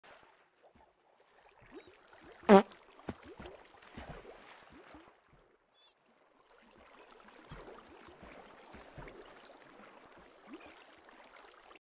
Быстрый способ изменить цвет
Меняем цвет зайца командой быстро и просто